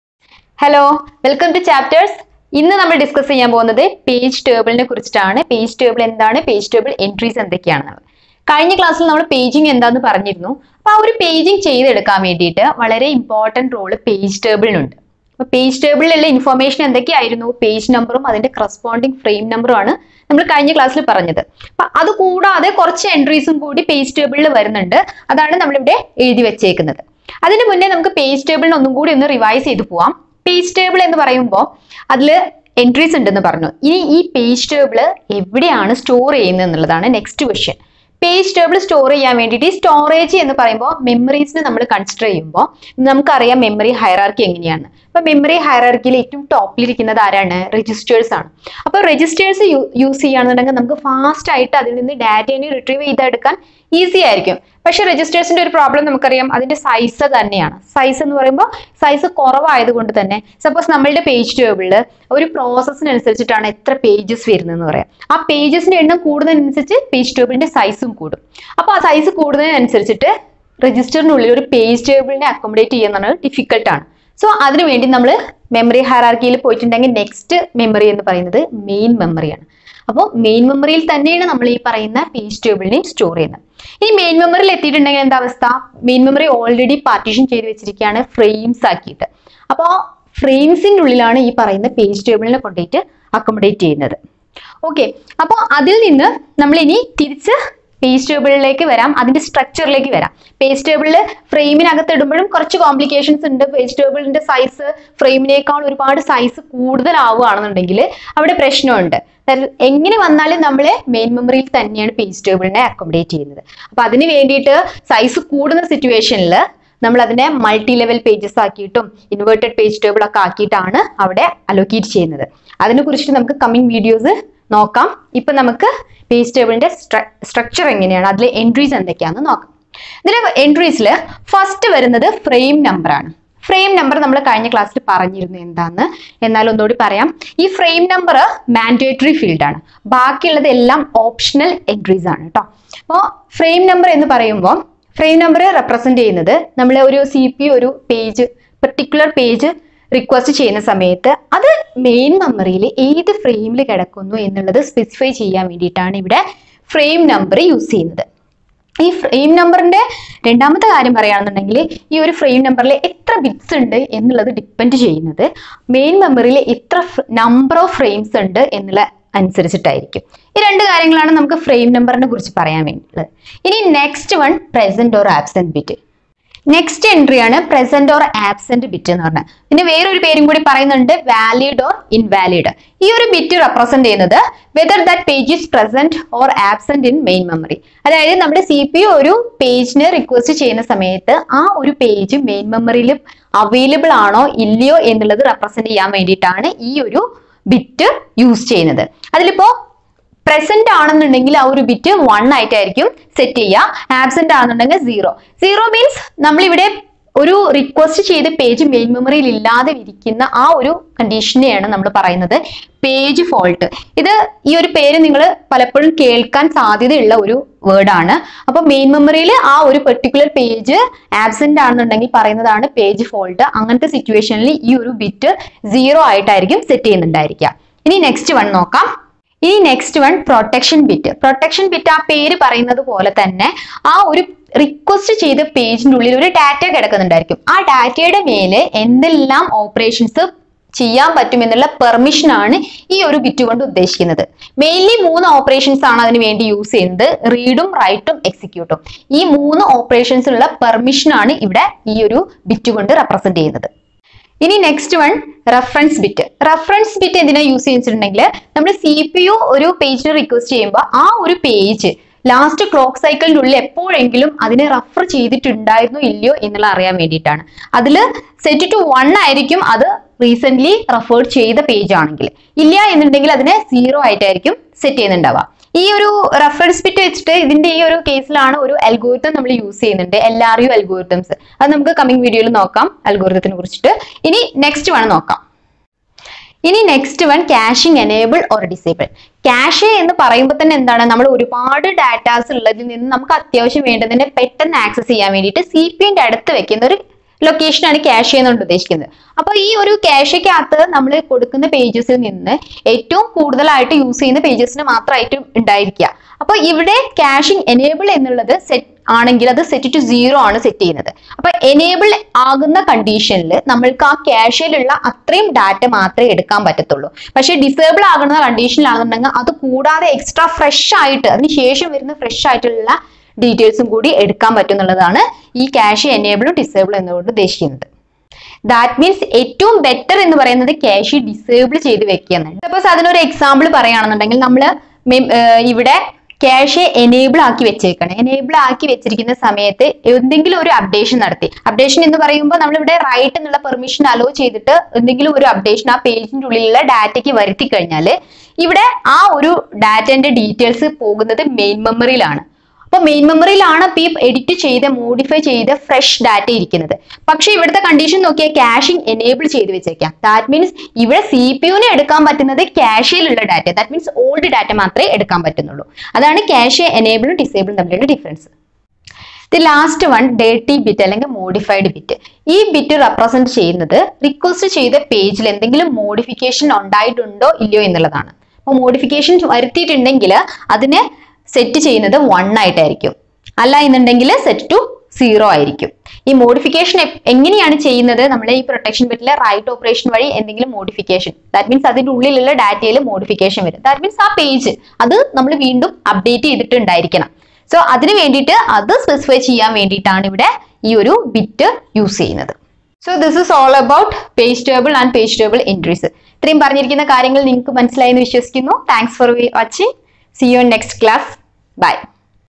PAGE TABLE ENTRY | DIRTY BIT | MEMORY MANAGEMENT | MALAYALAM | CHAPTERS
An overview about Page table entries and different components like  Frame number, present/absent, Protection, Reference, Caching, Dirty or Modified bit, Page fault By Reshma Prasoon
0:00 Introduction,basics of page table, where it is stored
2:10 structure of page table and entries of page table
3:00 present bit, absent bit
3:50 protection bit, reference bit,
4:50 cache enable and cache disable with example
6:10 dirty bit or modified bit